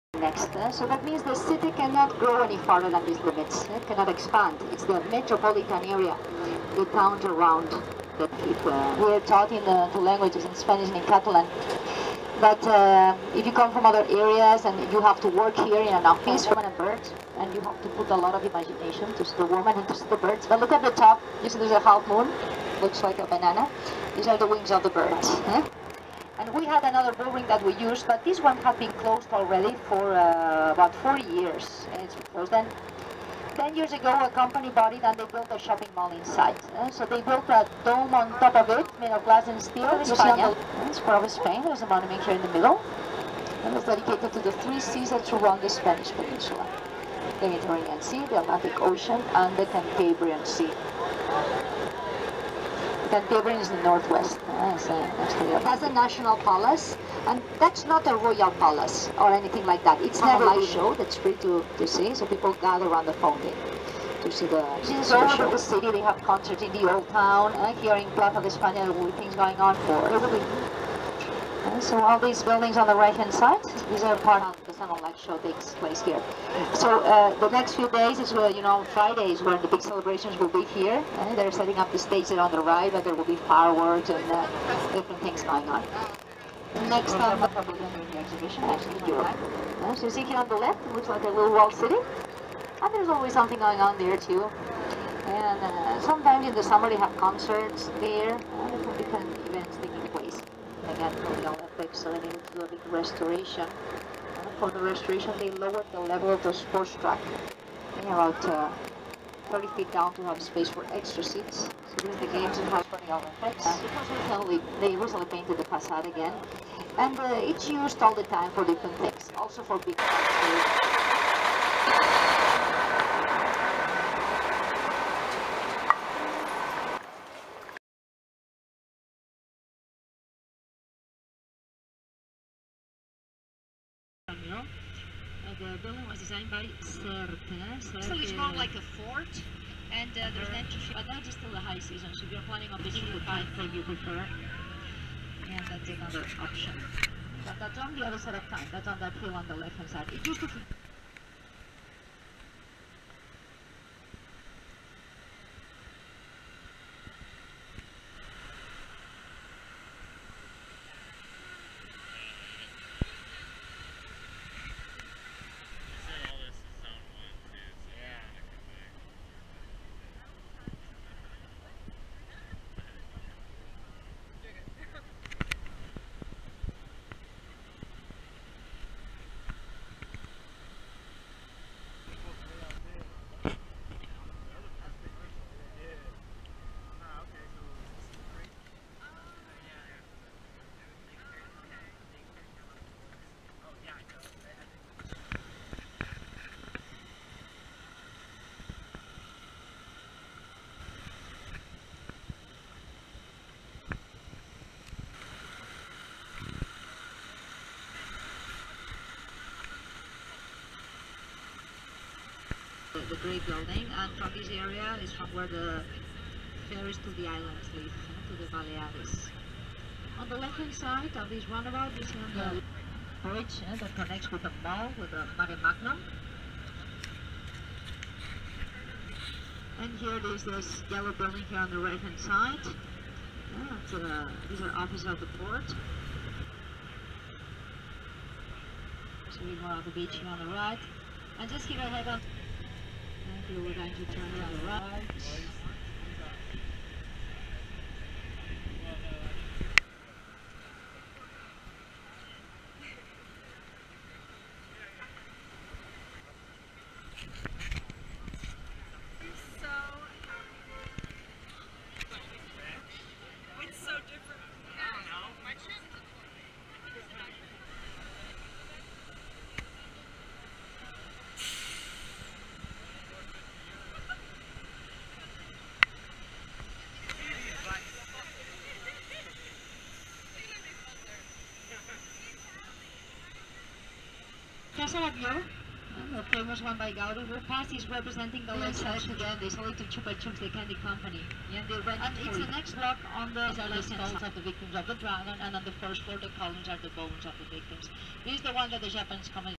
City Bus Tour
City bus tour of Barcelona, including some architecture, the olympic village, and other city sights